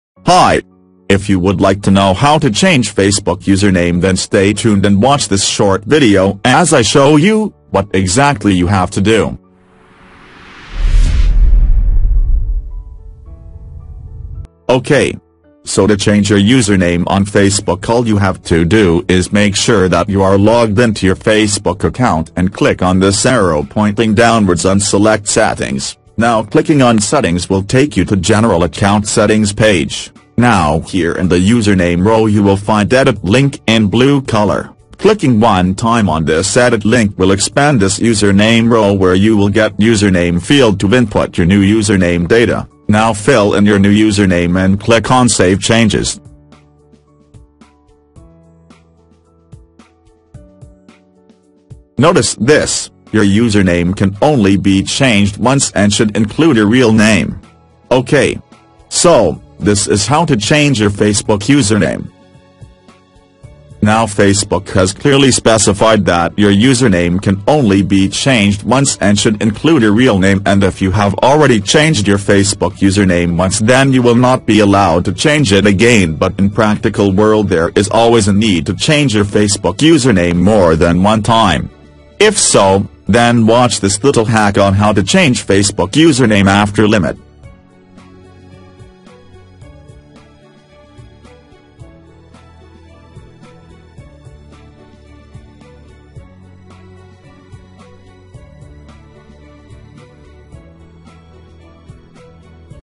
how to change facebook username 2015
how to change facebook username 2015 - Click to know how you can change facebook username.

4. Circle us on google plus

Video Contents:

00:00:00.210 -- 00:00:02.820
Hi!
If you would like to know “how to change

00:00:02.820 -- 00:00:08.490
this short video as I show you, what exactly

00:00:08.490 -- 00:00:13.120
you have to do?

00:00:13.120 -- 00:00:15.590
OK!
So, for demonstration purpose, here I have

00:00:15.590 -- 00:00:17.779
two facebook profile.
Ok! So to change your username on facebook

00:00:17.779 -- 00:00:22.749
all you have to do is make sure that you are
logged in to your facebook account and click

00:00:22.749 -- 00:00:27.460
on this arrow pointing downwards and select
“Settings”. Now clicking on “Settings”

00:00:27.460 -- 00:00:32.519
will take you to “General Account Settings”
page. Now here in the Username row you will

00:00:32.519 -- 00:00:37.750
find “Edit” link in blue colour. Clicking
one time on this “Edit” link will expand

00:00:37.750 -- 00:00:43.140
this UserName row where you will get “Username”
field to input your new Username data. Now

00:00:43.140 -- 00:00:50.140
fill in your new Username and click on save
changes.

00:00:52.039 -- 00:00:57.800
Notice this: Your username can only be changed
once and should include your real name.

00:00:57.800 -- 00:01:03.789
Ok! So, this is how to change your facebook
Username.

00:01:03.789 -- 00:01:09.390
Now facebook has clearly specified that “Your
username can only be changed once and should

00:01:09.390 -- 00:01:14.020
include your real name” &amp; if you have already
changed your facebook username once then you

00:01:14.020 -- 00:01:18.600
will not be allowed to change it again but
in practical world there is always a need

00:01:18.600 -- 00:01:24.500
to change your facebook username more than
one time. If so, then watch this little hack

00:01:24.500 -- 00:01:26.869
on how to change facebook username after limit.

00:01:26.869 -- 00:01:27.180
&amp; twitter profile to let your friends &amp; family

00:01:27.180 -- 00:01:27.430
know about it.

Incoming search terms:

how to change facebook username
change facebook username